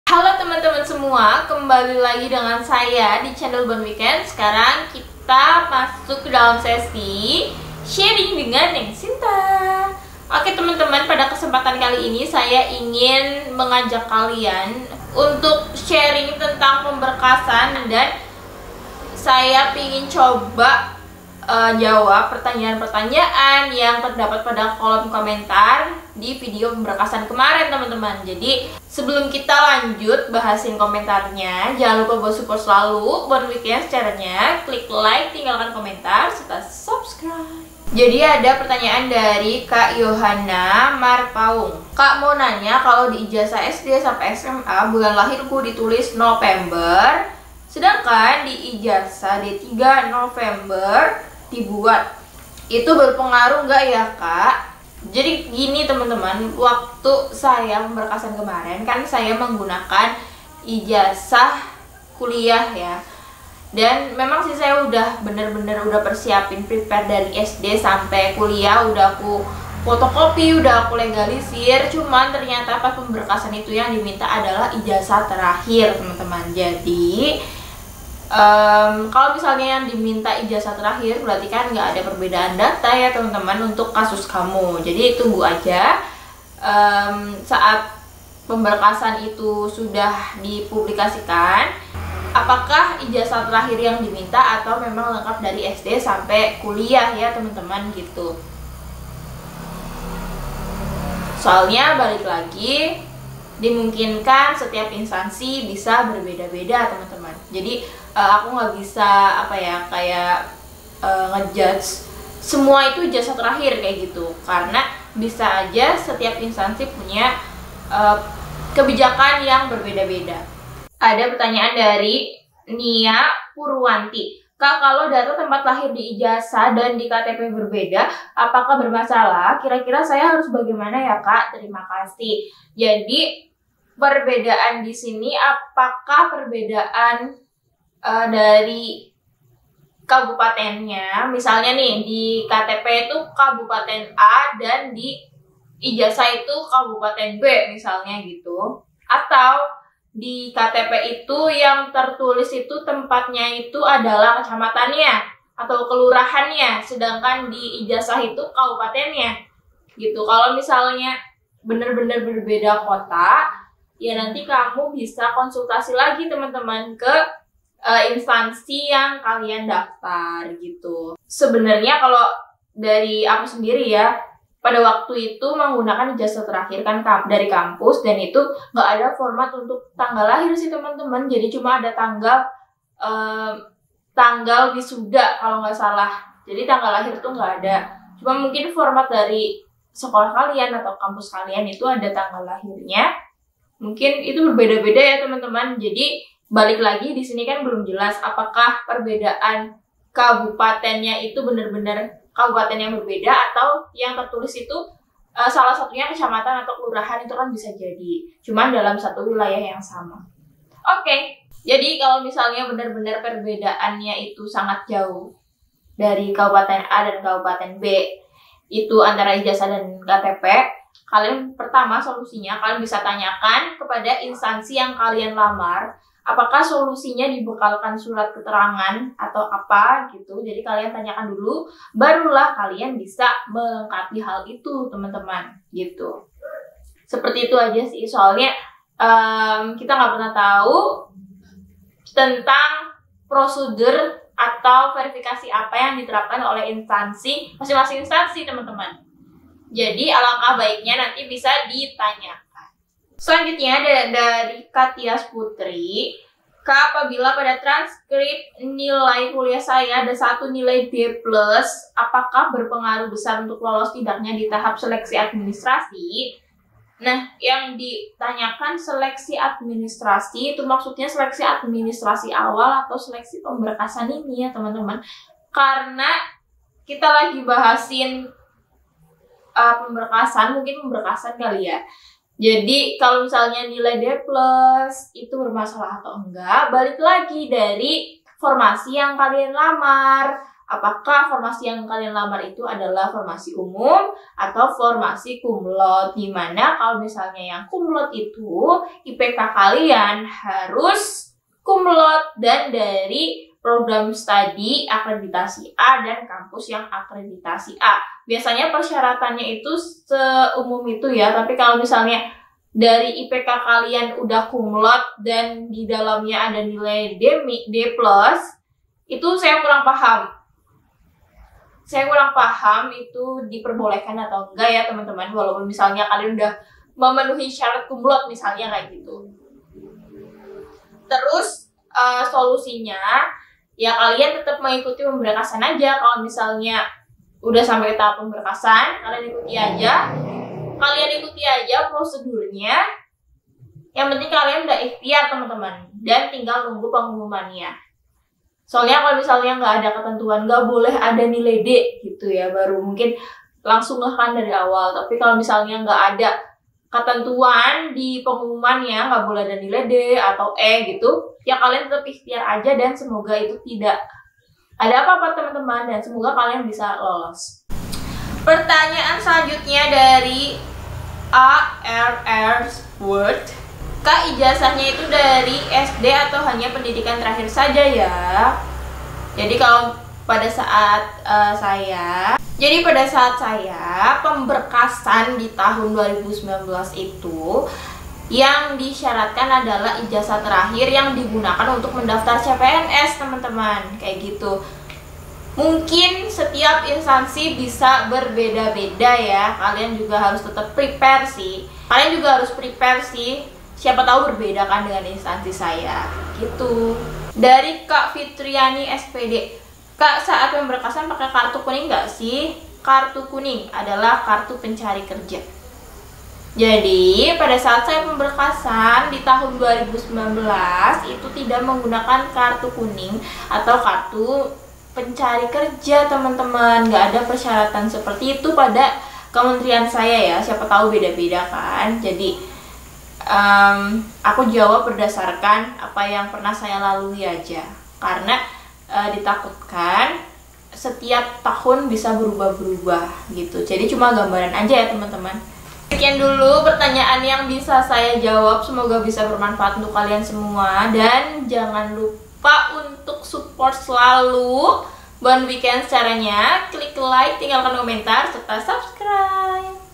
Menjawab pertanyaan tentang pemberkasan cpns!
Hallo teman-teman! Bagaimana kabarnya? Saya ucapkan selamat untuk kalian semua yang masuk ke tahap pemberkasan cpns yaa.. Pada video ini saya ingin mencoba menjawab pertanyaan kalian tentang pemberkasan.. Semoga bermanfaat